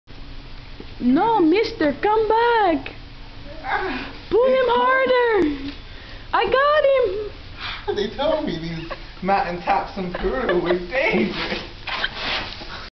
Boy Falls Off a Cliff in Peru
Spanish Class Peru Trip